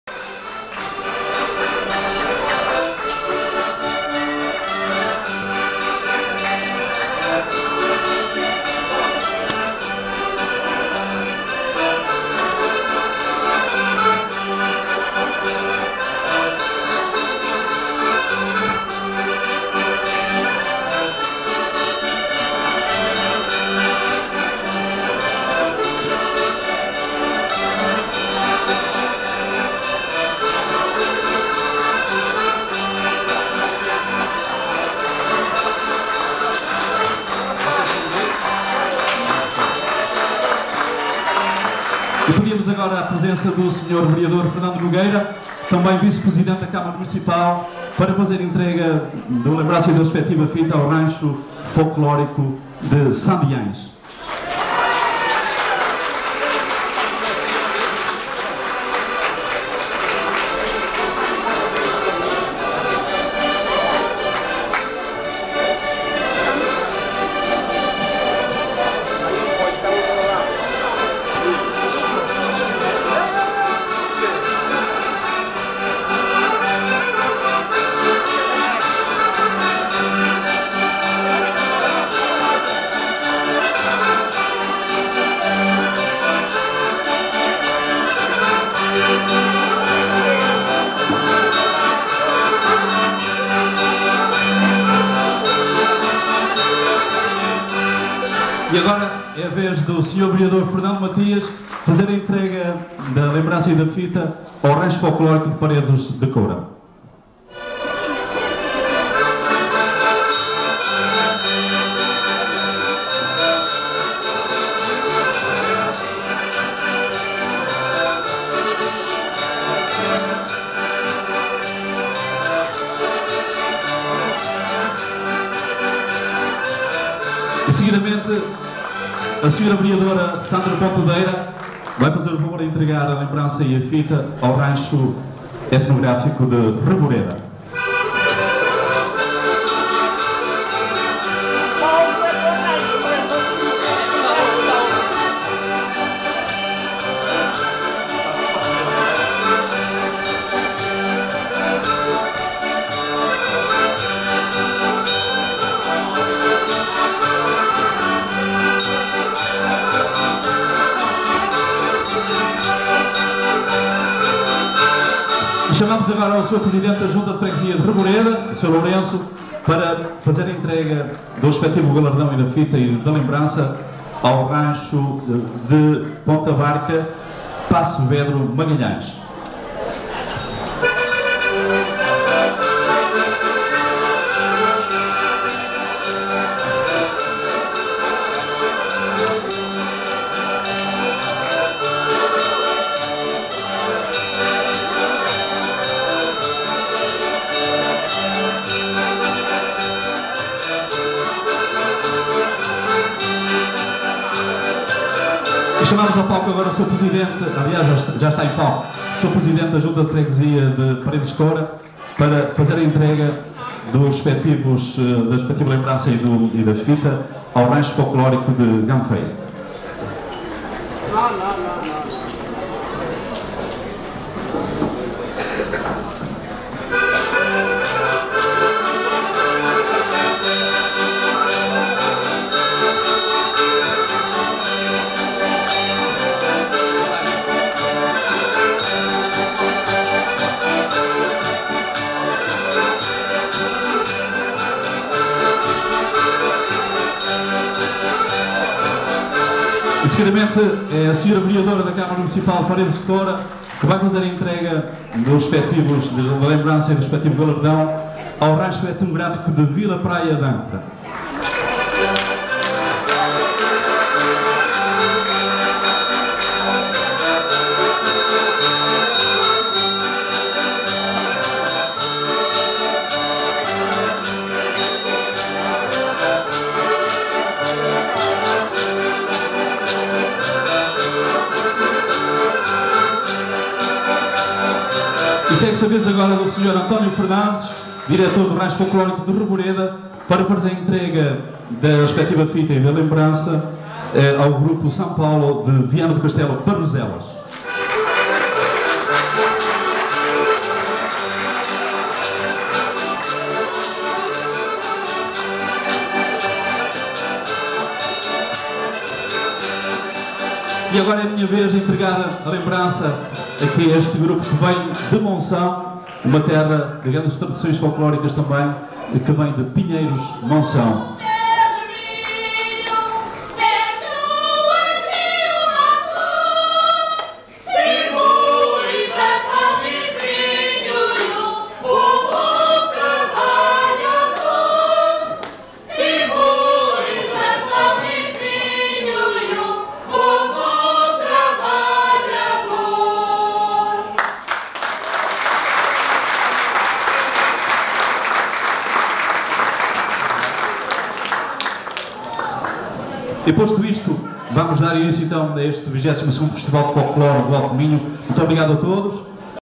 01--22º FESTIVAL FOLCLÓRICO DISTRTAL ALTO MINHO V.N.CERVEIRA-17.06.2012
22º FESTIVAL FOLCLÓRICO DISTRTAL ALTO MINHO V.N.CERVEIRA-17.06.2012
ENTREGA DE LEMBTANÇAS AOS GRUPOS PARTICIPANTES.